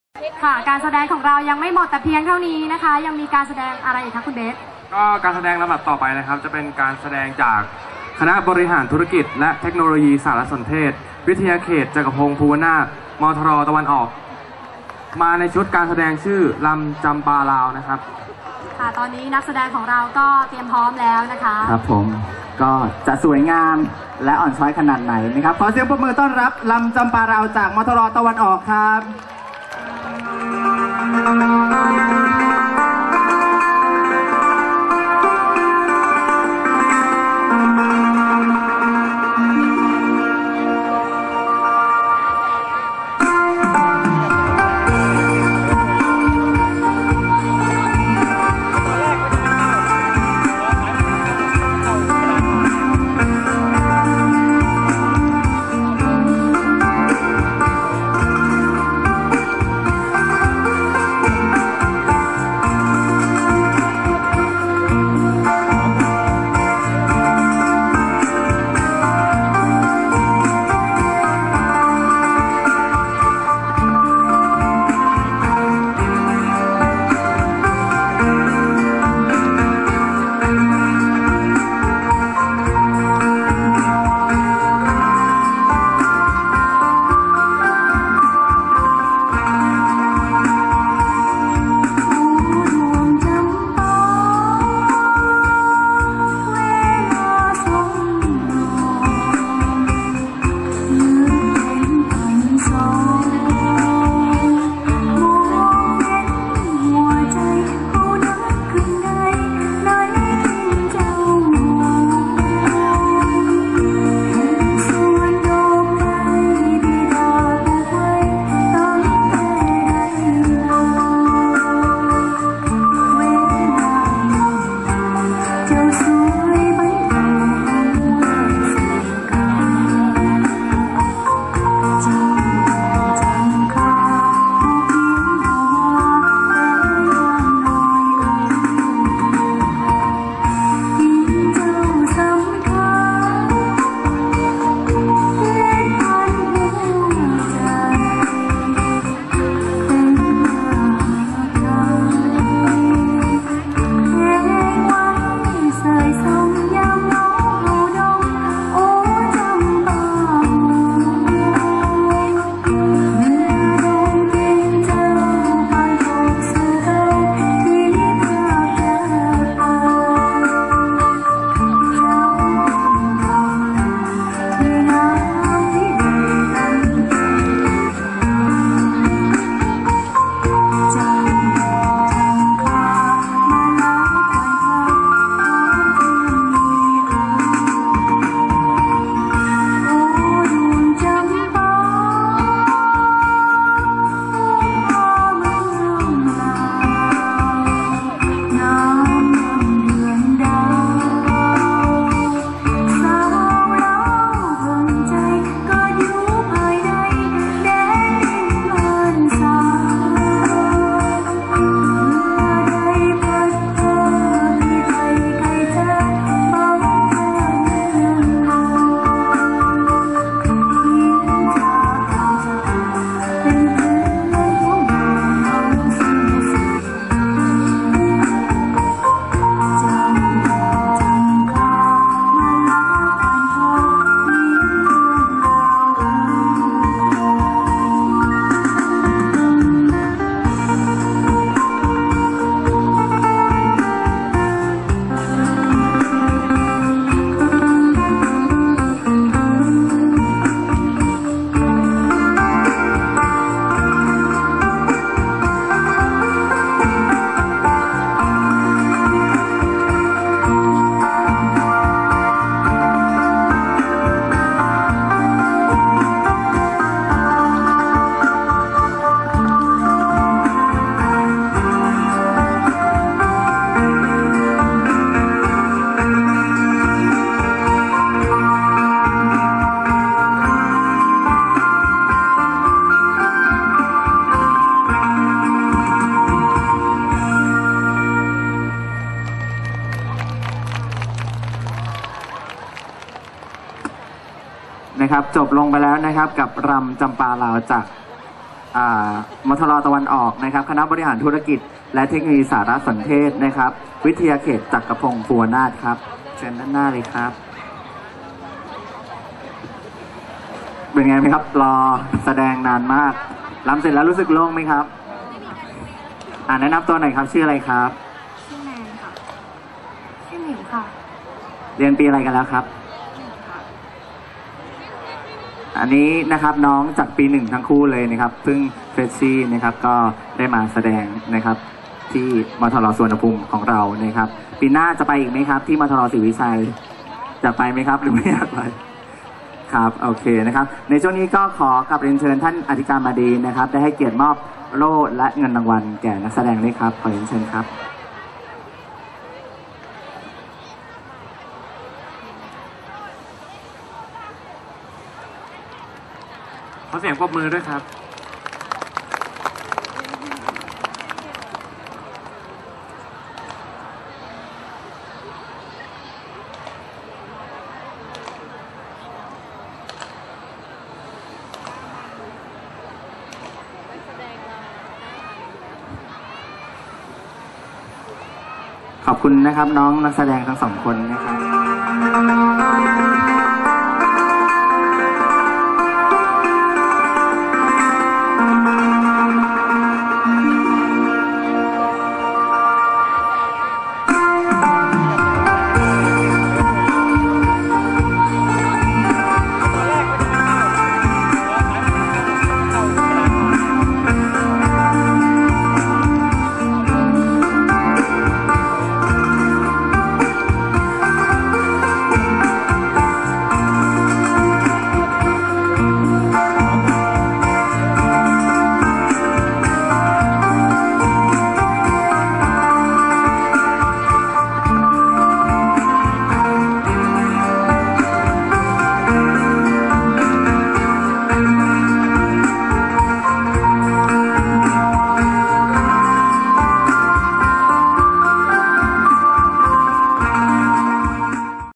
การแสดงของมทรตะวันออกวิทยาเขตจักรพงษภูวนารถ
การสัมนาและการแข่งขันทักษะด้านวิชาการและด้านบริหารธุรกิจ 9 มทร ครั้งที่ 2 และ
27-29 พฤศจิกายน 2556
งานสืบสานศิลวัฒนธรรมครั้งที่5
สาขาวิชาการตลาด 26-28 พฤศจิกายน 2556